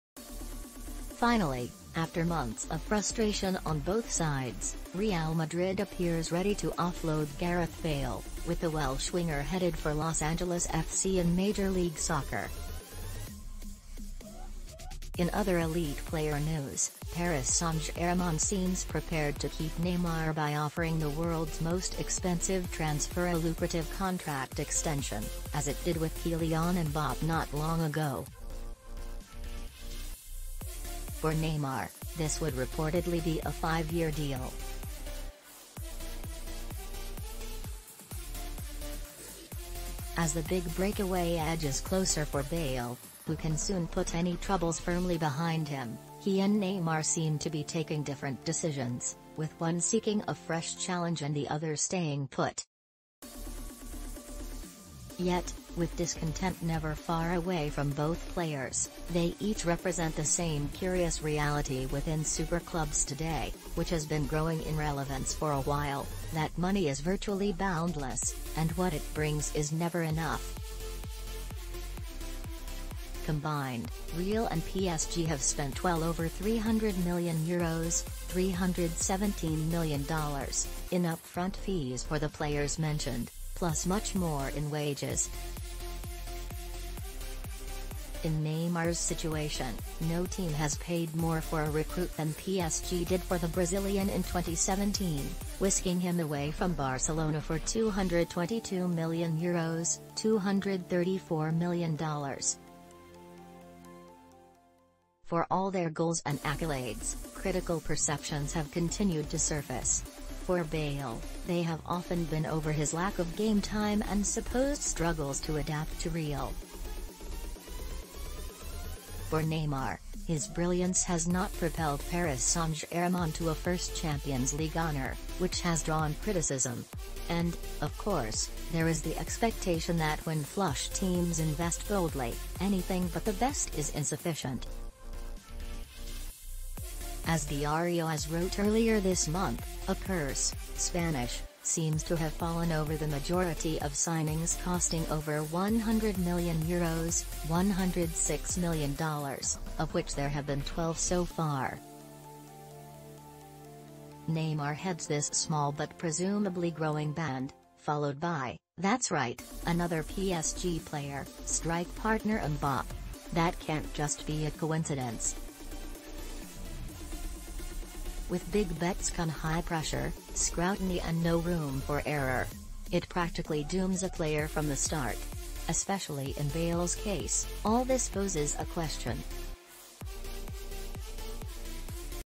Gareth Bale And Neymar Have Different Plans; Both Reflect The Same Issue
Finally, after months of frustration on both sides, Real Madrid appears ready to offload Gareth Bale, with the Welsh winger headed for Los Angeles FC in Major League Soccer. In other elite-player news, Paris Saint-Germain seems prepared to keep Neymar by offering the world’s most expensive transfer a lucrative contract extension, as it did with Kylian Mbappé not long ago. For Neymar, this would reportedly be a five-year deal. As the big breakaway edges closer for Bale, who can soon put any troubles firmly behind him, he and Neymar seem to be taking different decisions, with one seeking a fresh challenge and the other staying put. Yet, with discontent never far away from both players, they each represent the same curious reality within super clubs today, which has been growing in relevance for a while—that money is virtually boundless, and what it brings is never enough. Combined, Real and PSG have spent well over €300 million ($317 million) in upfront fees for the players mentioned, plus much more in wages. In Neymar’s situation, no team has paid more for a recruit than PSG did for the Brazilian in 2017, whisking him away from Barcelona for €222 million ($234 million). For all their goals and accolades, critical perceptions have continued to surface. For Bale, they have often been over his lack of game time and supposed struggles to adapt to Real. For Neymar, his brilliance has not propelled Paris Saint-Germain to a first Champions League honor, which has drawn criticism. And, of course, there is the expectation that when flush teams invest boldly, anything but the best is insufficient. As Diario AS wrote earlier this month, a curse (Spanish) seems to have fallen over the majority of signings costing over €100 million ($106 million), of which there have been 12 so far. Neymar heads this small but presumably growing band, followed by—that’s right, another PSG player—strike partner Mbappé. That can’t just be a coincidence. With big bets come high pressure, scrutiny and no room for error. It practically dooms a player from the start. Especially in Bale’s case, all this poses a question. What, exactly, do we expect money to bring? If trophies are the answer, Bale—admirer or not—has ticked all the boxes. Indeed, his performances have clinched final victories in the Champions League and domestic Copa del Rey, with him scoring some of the best Madrid goals ever. When he leaves, those will stay intact. It’s crazy to consider that some will be relieved to see such a player leave. What’s also crazy is how Real has gone on to win more trophies with him on the sidelines, a peripheral figure to whom they can still afford to pay high wages. Equally, the idea that Bale has gone from a hero to a problem is bizarre. Unlike Bale, who did so for a while, Neymar has not become the ultimate game-changer many would have expected when he left Barcelona for Paris. He has brought something to PSG.